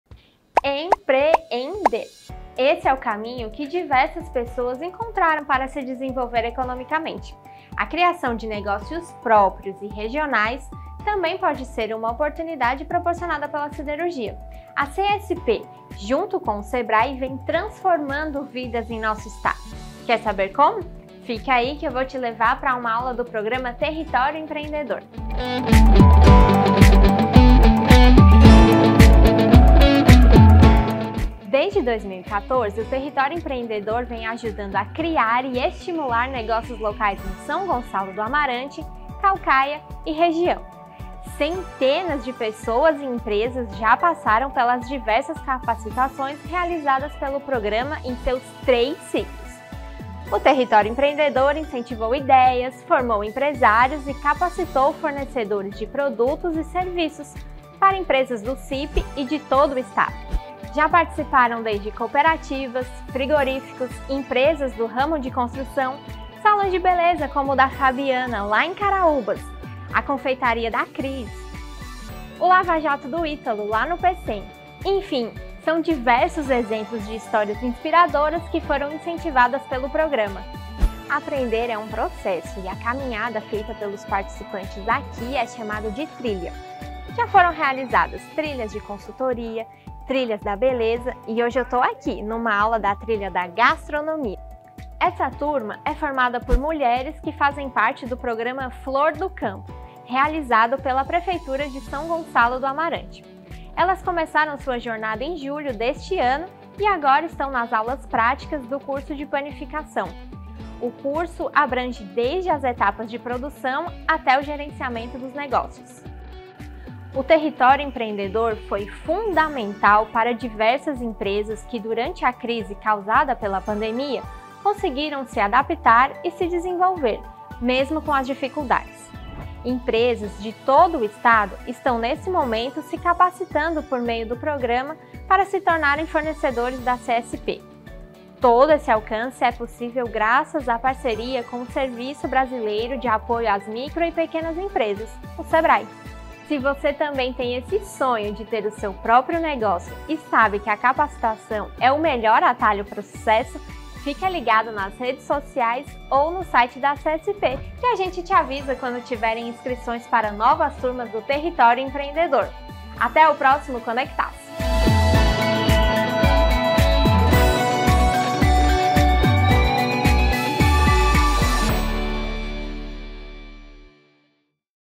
Saiba como o Território Empreendedor tem promovido oportunidades no Ceará | Conectaço
Conhece alguém que empreende ou já quis ser empreendedor? 🤑 Neste episódio do Conectaço você vai conhecer um importante programa da CSP para capacitar e estimular empreendedores locais. ▶️

🤝 Em parceria com o Sebrae-CE, o Território Empreendedor já tornou possível a realização de muitos sonhos através das suas trilhas. Já foram realizadas trilhas da beleza, consultoria, gastronomia e muitas outras.

É importante acrescentar que esta capacitação está sendo vital durante a pandemia que está desafiando os empreendedores.

⚠️ Quer conhecer mais como o Território Empreendedor está mudando a história de vida de muitos cearenses? Acesse agora o site da CSP: cspecem.com

CSP